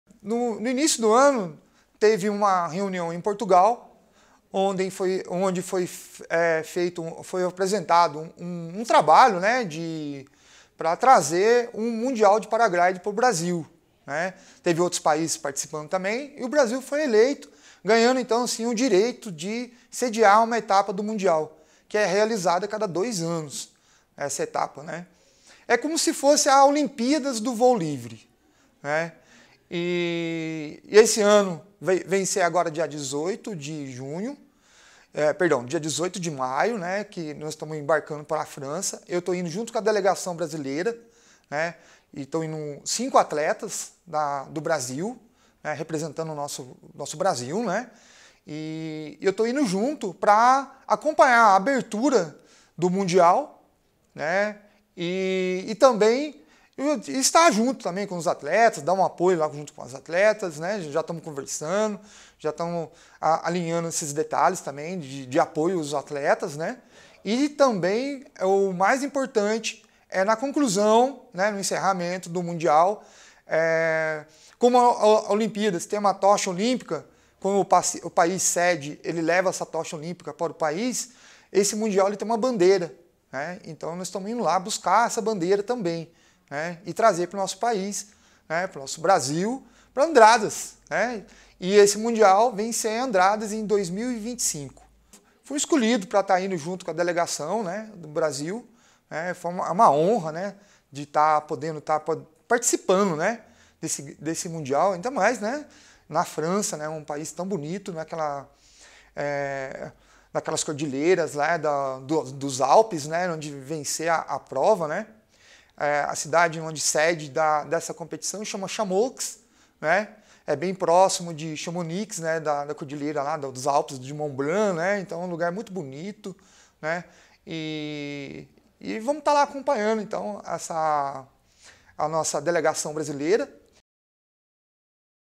Coordenador do Projeto Pro-Vida participa da abertura do Mundial de Parapente na França
Supervisor do projeto Pró-Vida está entre os brasileiros que participam dos eventos relacionados ao Campeonato Mundial de Paraglider que acontece na França.